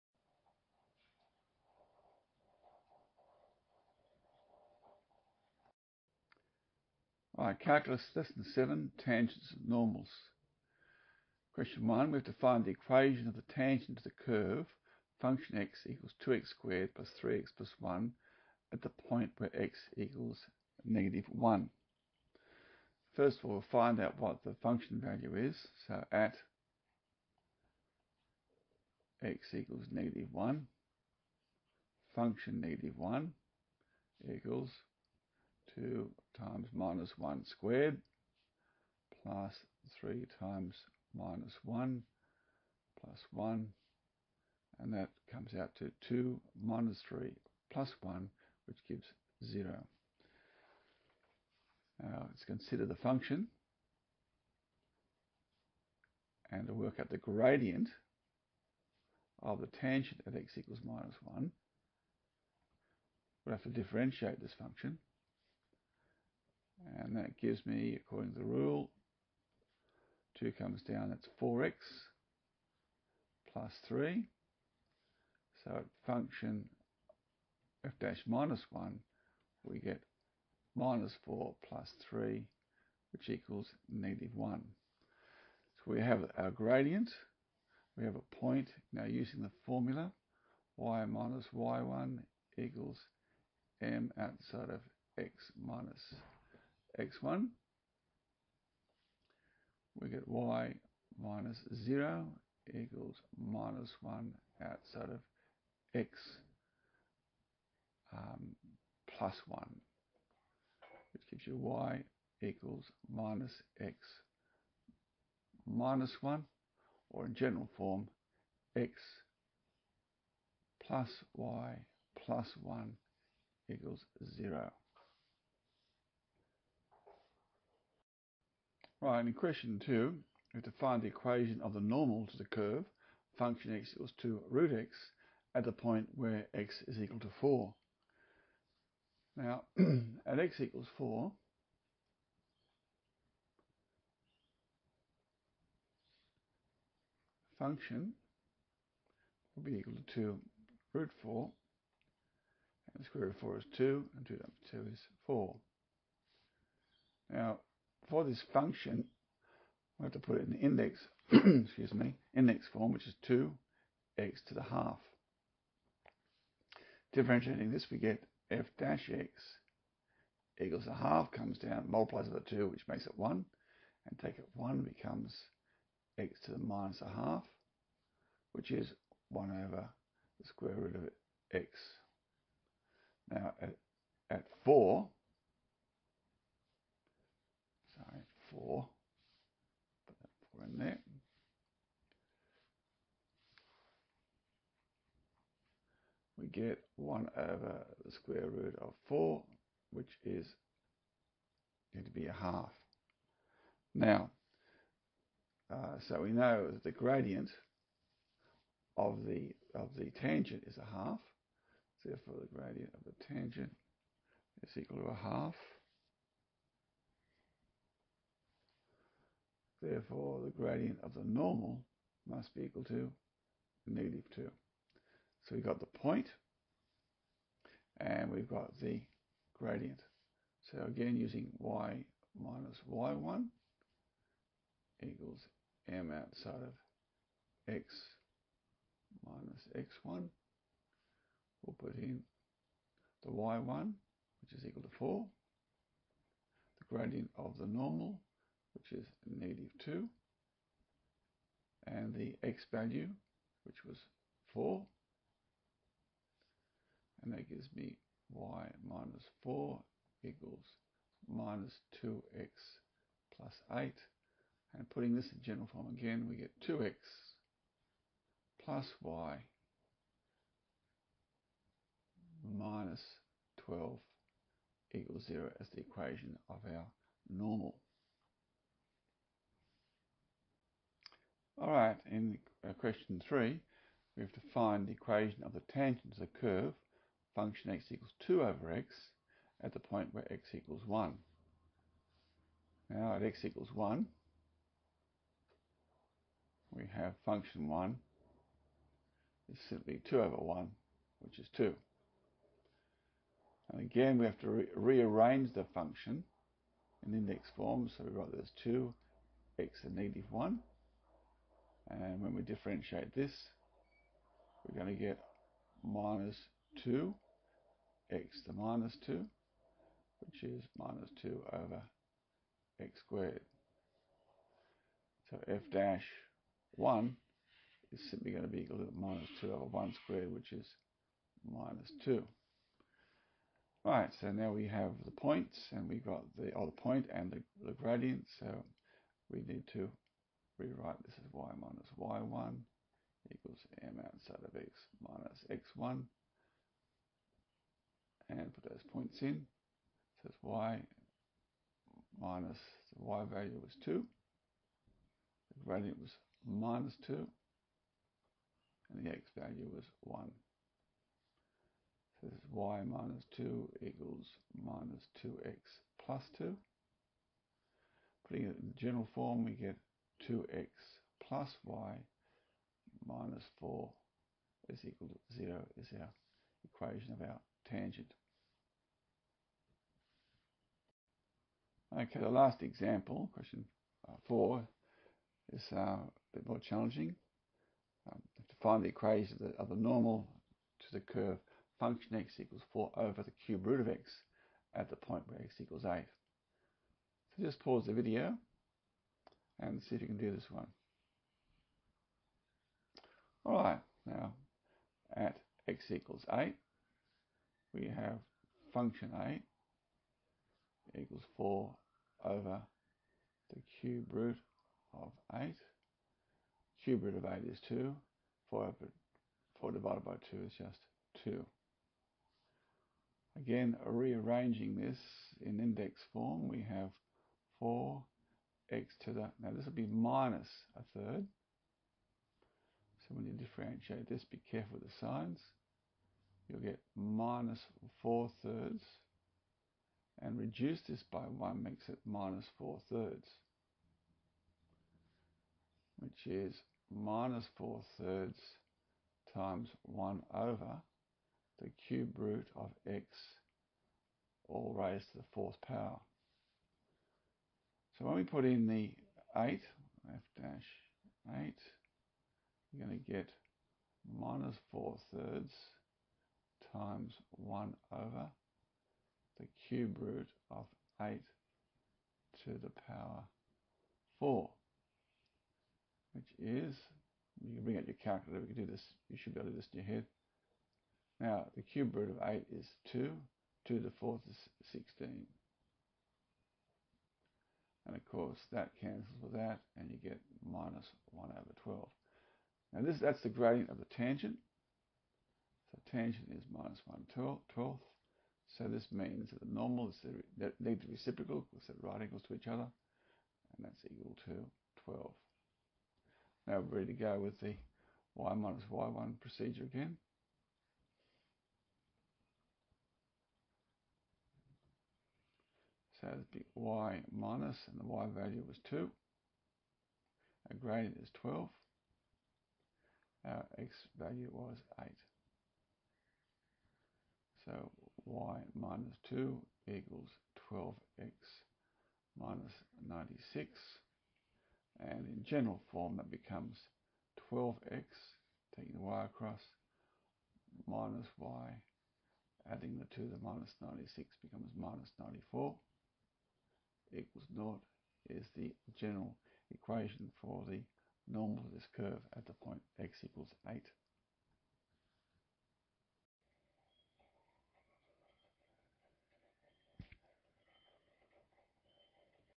Calculus 1 Lesson 7: Tangents and Normals
Calculus is applied to determine the equations of tangents and normals to a variety of functions.